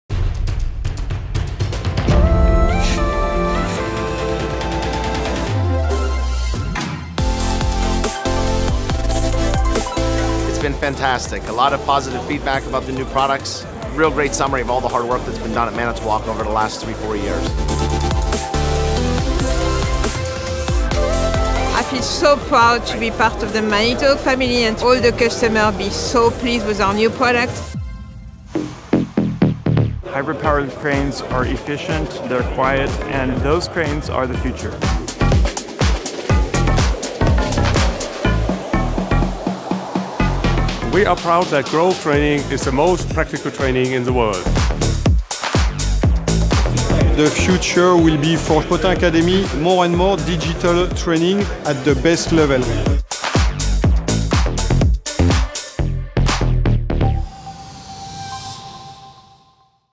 bauma 2022 -- Day 5 Recap
Day 5 done! Another busy day at the Manitowoc booth at bauma2022 and we caught up with a couple of members of our leadership team to get their feedback on the show. We also took a closer look at our exciting plug-in hybrid prototype of a Grove GMK4100L-2 plus we were “talking training” in our customer center, showing how the Potain eAcademy and Grove mobile training programs point to a bright future for crane owners, operators, and technicians. Thanks to everyone who stopped by our booth.